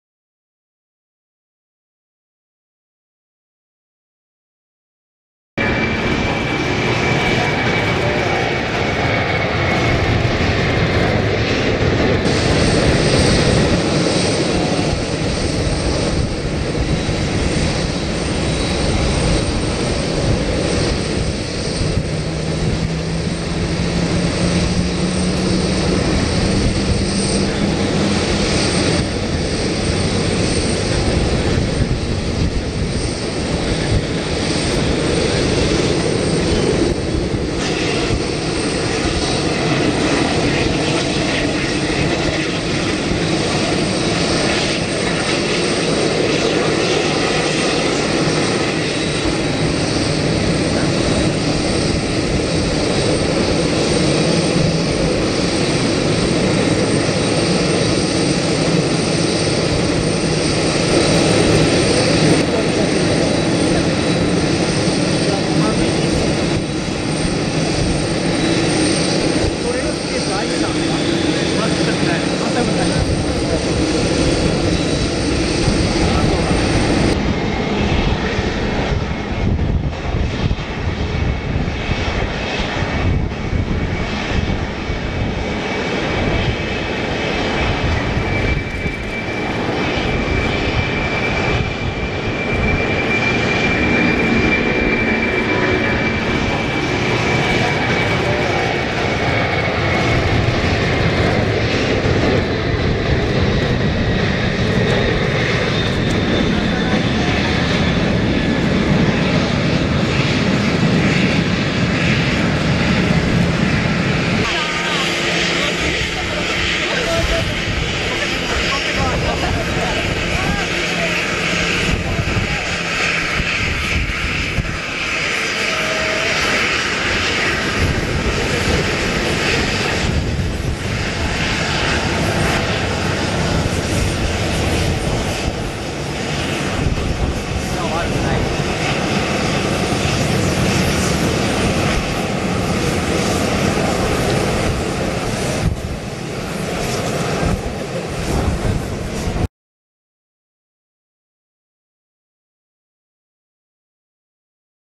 ✈✈IRON MAIDEN アイアン・メイデン 5月に4年振りの来日(Air Atlanta Icelandic) Boeing 747-428 TF-AAK 羽田空港
アイアン・メイデン 4年振りの来日決定「LEGACY OF THE BEAST TOUR」
2016/4/22に撮影したものです。
エア アトランタ アイスランド
(Air Atlanta Icelandic)
Boeing 747-428
TF-AAK